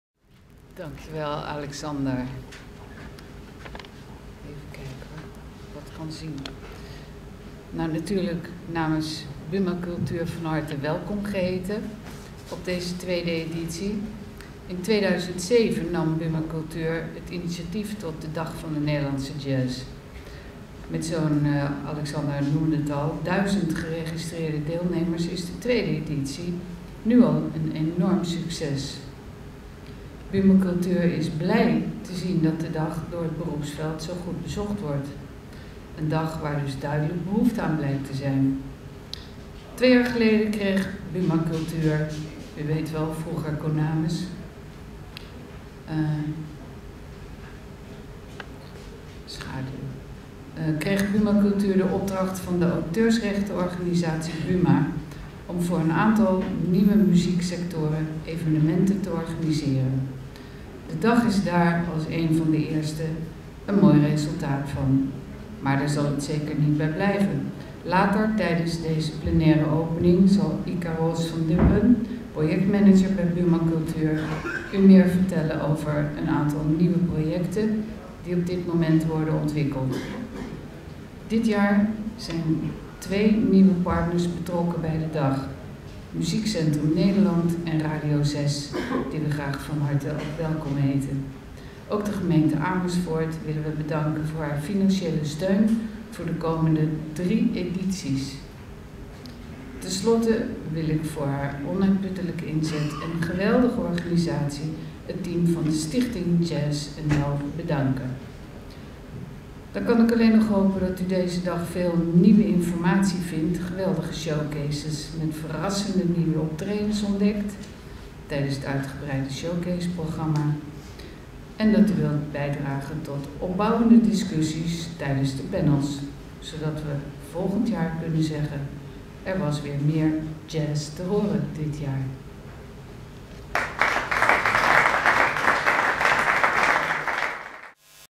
JAZZDAG.NL - Keynote Speech / Jerney Kaagman Buma Cultuur
Beschermvrouwe Jerney Kaagman spreekt tijdens de Dag van de Nederlandse Jazz.
Amersfoort 16 mei 2008.